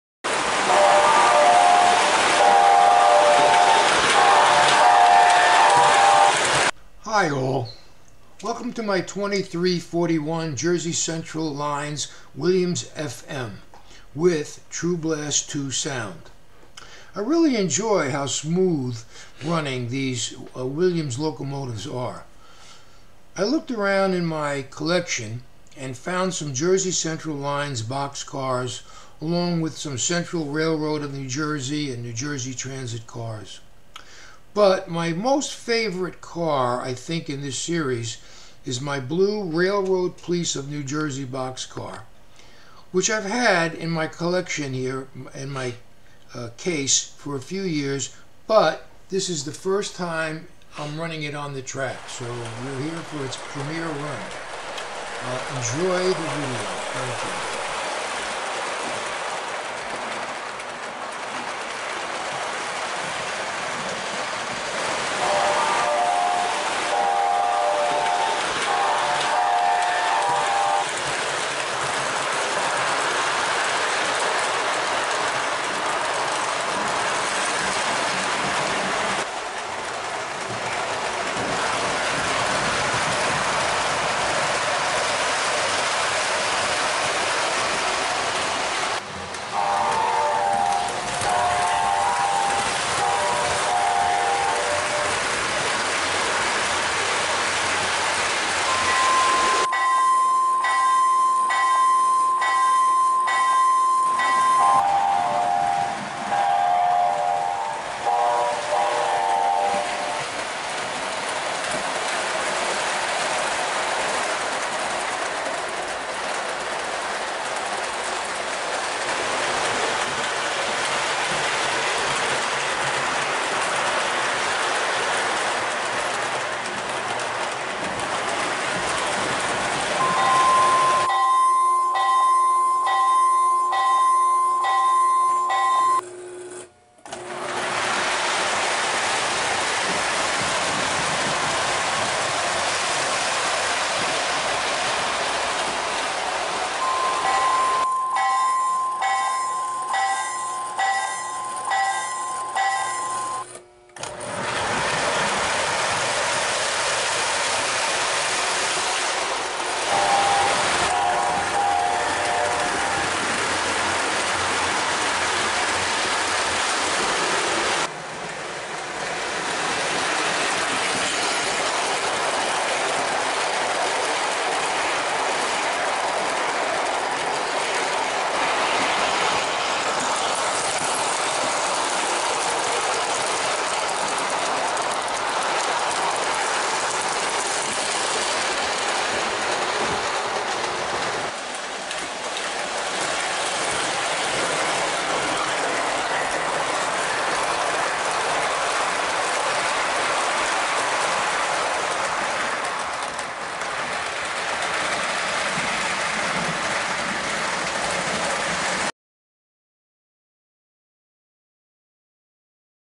Williams 2341 Jersey Central Lines FM 102 Trainmaster
Williams 2341 O Gauge FM Fairbanks Morse 102 Trainmaster Jersey Central Lines running with True Blast II Horn and Bell sounds. Consist composed of Jersey Central Lines, Central Railroad of NJ, NJ Transit tank car and Railroad Police of NJ boxcars. First time run for the blue Railroad Police of NJ boxcar.